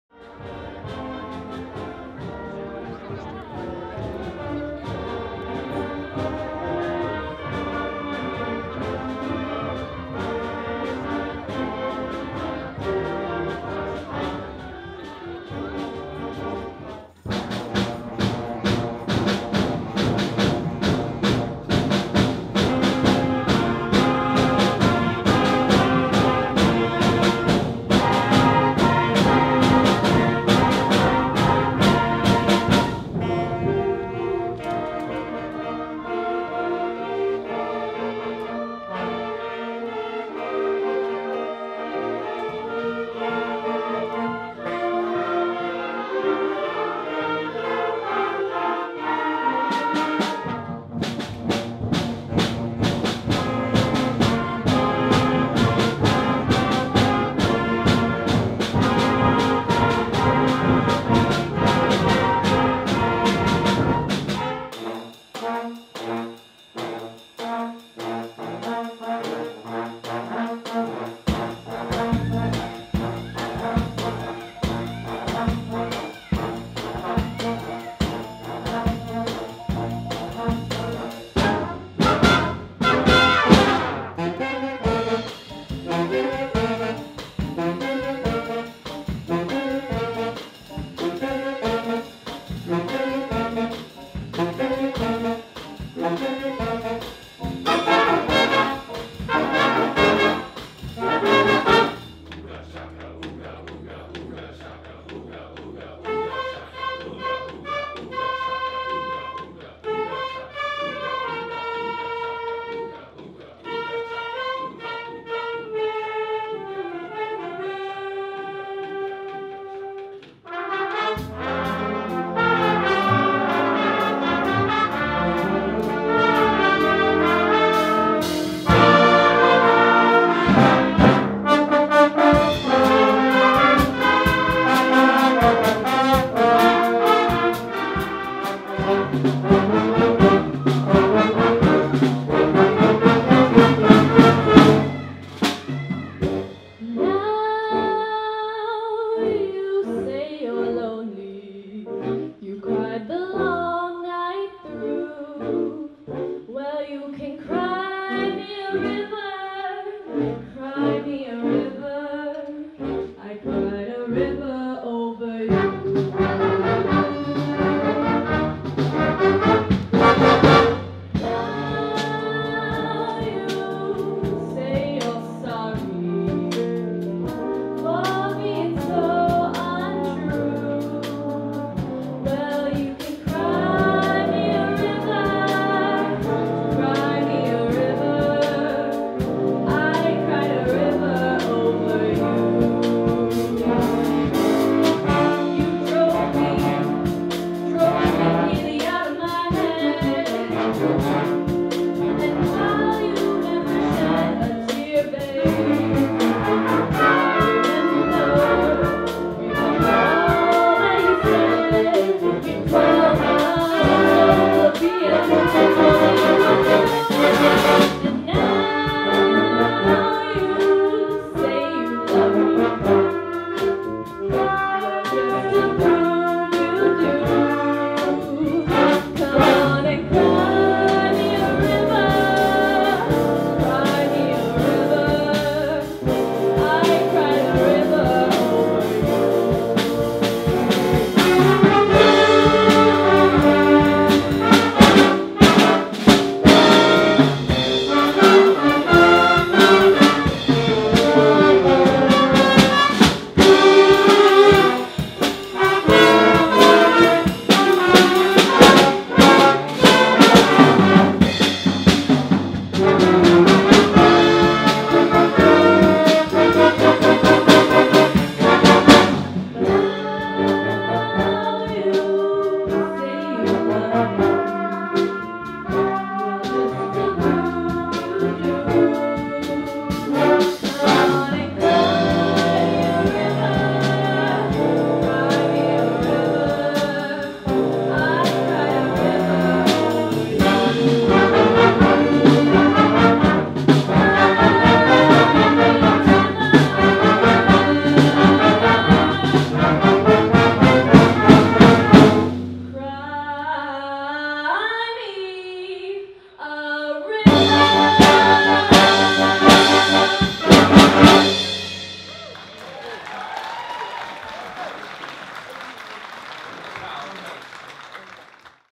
Year-End Band Concert in Beacon Hill Park
The Glenlyon Norfolk School Middle and Senior School Concert and Jazz Bands celebrated the end of the school year by performing an outdoor concert at Beacon Hill Park's Cameron Bandshell on June 1, 2022.

Some light rain didn't dampen any spirits, as their music was enjoyed by friends and family in attendance and heard throughout the park!

Glenlyon Norfolk School is an independent JK to Grade 12 International Baccalaureate and Round Square day and boarding school in Victoria, BC Canada.